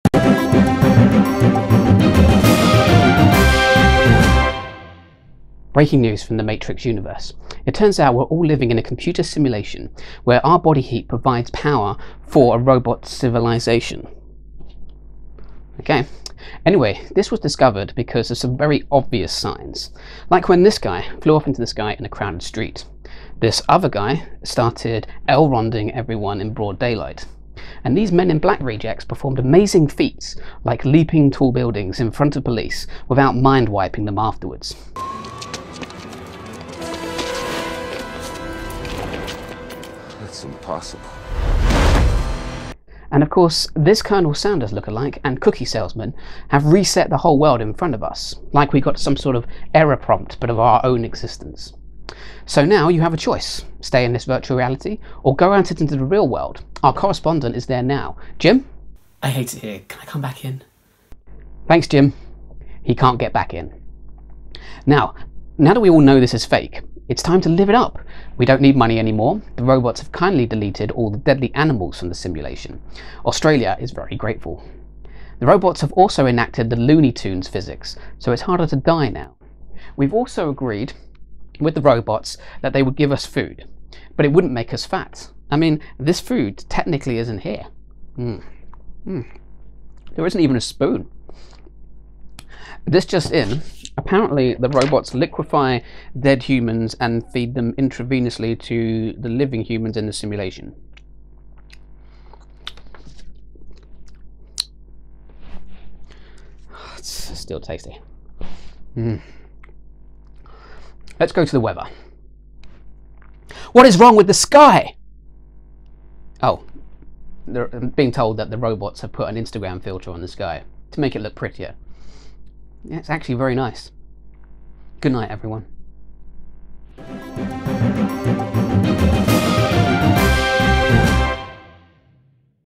Honest Movie News - The Matrix Universe
Fed up of the News? Well why not try the news from some of your favourite Movie Universes.

This week the news from The Matrix universe. Check out the follow up video where The 8th Plot explores alternative story ideas based on the What If? question this film asks.